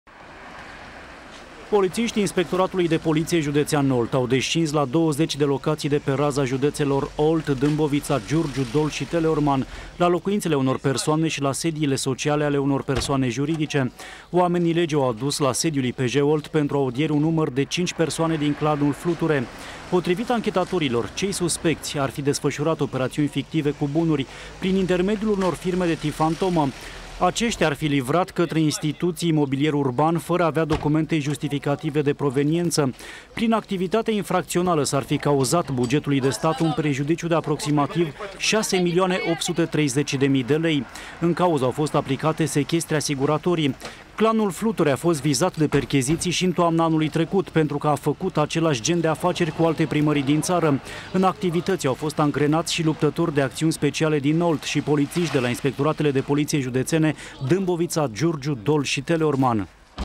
ZECI DE PERCHEZIȚII ÎN CINCI JUDEȚE
Poliţiştii din Olt au efectuat, în această dimineaţă, 20 de percheziţii, în 5 judeţe, pentru documentarea activităţii infracţionale a unor persoane acuzate de evaziune fiscală și spălare de bani. Potrivit anchetatorilor, acestea ar fi derulat operațiuni fictive de bunuri prin intermediul unor firme fantomă. Prejudiciul cauzat bugetului de stat prin evaziune fiscală și spălare de bani este de aproape şapte milioane de lei. Cinci persoane au fost aduse la audieri la sediul IPJ Olt.